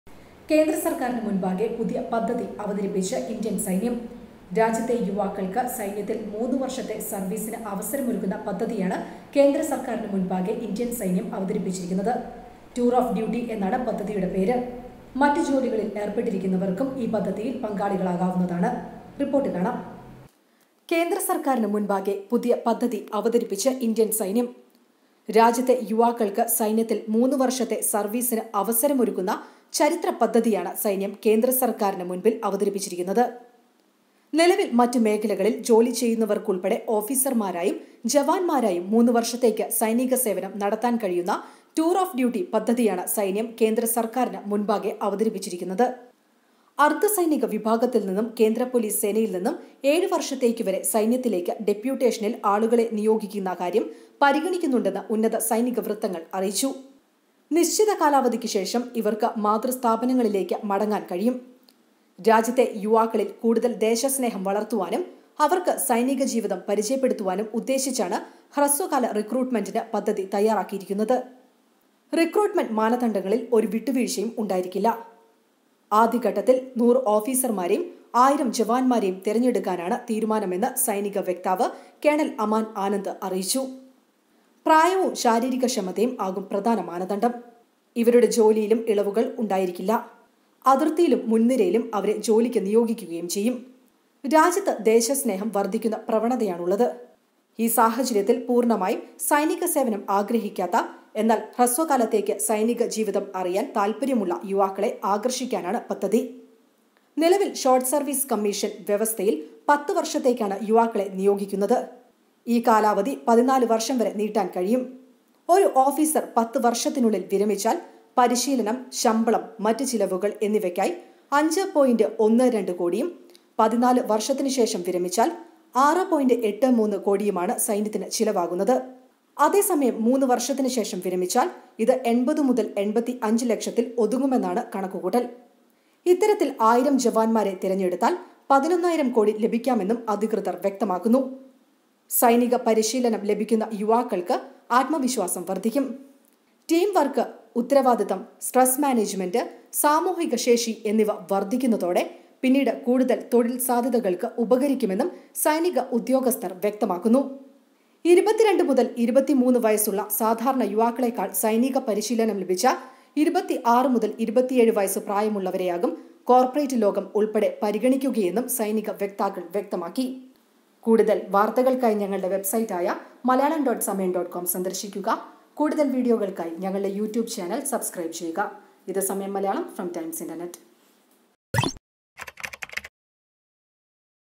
ചരിത്ര പദ്ധതി അവതരിപ്പിച്ച് ഇന്ത്യൻ സൈന്യം | Samayam Malayalam
The Army is considering a proposal to allow civilians, including young working professionals, to join the force for three years as officers and in other ranks in areas like logistics and front-line formations.The Army is also considering to take personnel from the paramilitary and central armed police forces for up to seven years, after which they would be allowed to return to their parent organisations, military sources have said.

കേന്ദ്ര സർക്കാരിന് മുമ്പിൽ പുതിയ പദ്ധതി അവതരിപ്പിച്ച് ഇന്ത്യൻ സൈന്യം. രാജ്യത്തെ യുവാക്കള്‍ക്ക് സൈന്യത്തില്‍ മുന്നു വര്‍ഷത്തെ ഹ്രസ്വകാല സര്‍വീസ് അവസരമൊരുക്കുന്ന ചരിത്രപദ്ധതിയാണ് സൈന്യം കേന്ദ്രസർക്കാരിന് മുമ്പിൽ അവതരിപ്പിച്ചിരിക്കുന്നത്. നിലവില്‍ മറ്റ് മേഖലകളില്‍ ജോലി ചെയ്യുന്നവര്‍ക്കുള്‍പ്പെടെ ഓഫിസര്‍മാരായും ജവാന്മാരായും മൂന്നു വര്‍ഷത്തേക്ക് സൈനിക സേവനം നടത്താന്‍ കഴിയുന്ന ടൂര്‍ ഓഫ് ഡ്യൂട്ടി (ടിഒഡി) പദ്ധതിയാണ് സൈന്യം കേന്ദ്ര സര്‍ക്കാരിനു മുന്നില്‍ അവതരിപ്പിച്ചിരിക്കുന്നത്.